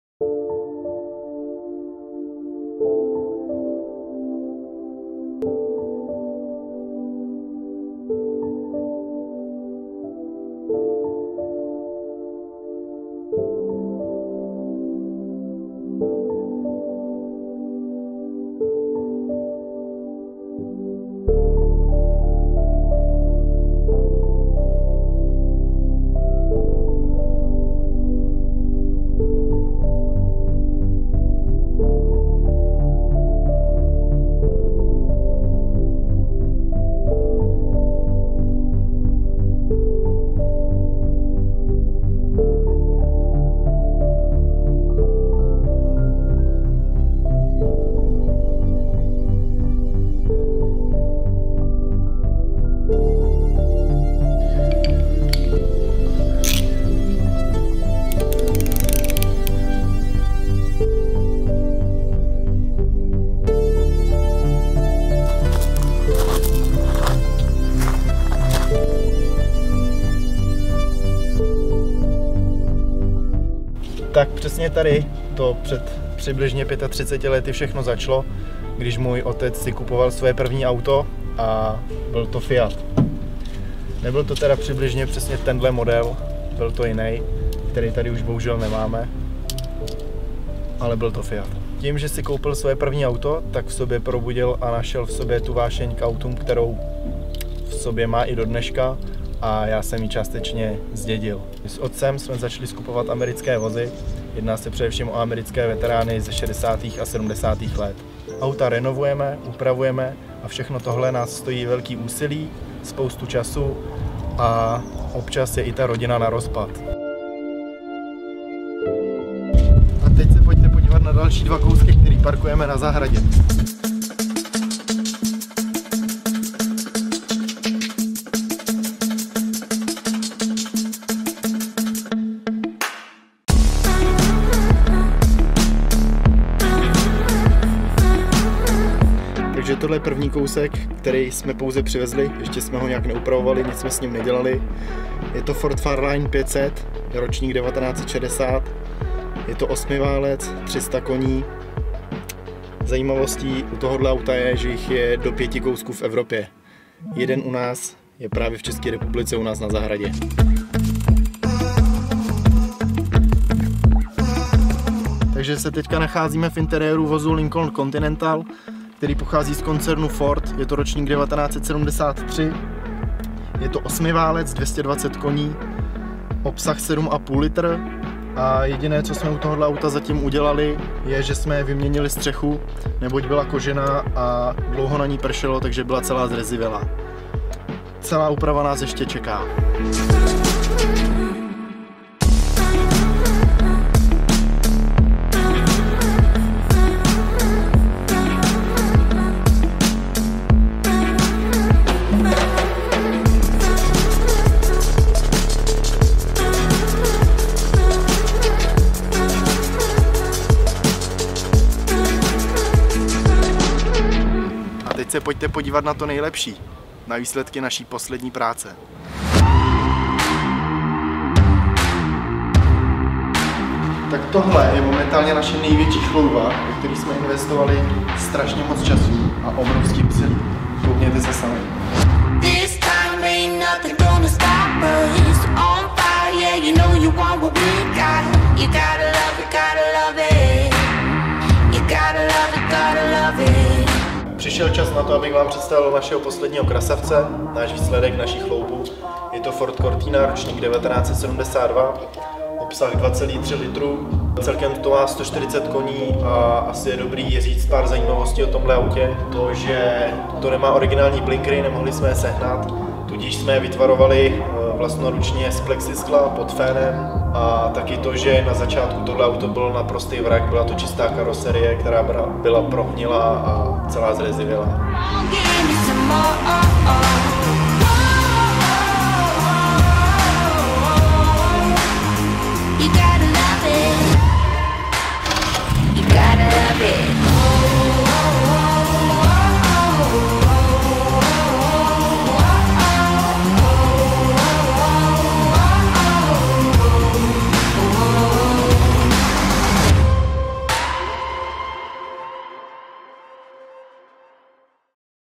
US CARS - Michal Mařík
US old Cars are big passion of Michal and his father. I helped them to catch their hard work on all the cars they have made. It was something new for me, but great experience to film it.

When I saw interior of cars from 60s and 70s, I was wondered of all the car equipment, mainly of automatic gearbox and air conditioning. Great example how Europe was behind US.

EQUIPMENT USED:
Nikon D550O
Nikon 20mm f/1,8 G ED
Nikon AF-P 70-300mm/4.5-6.3G ED
Neewer Carbon Fiber 24"/60cm

EDITED:
Premiere Pro
After Effects
Lightroom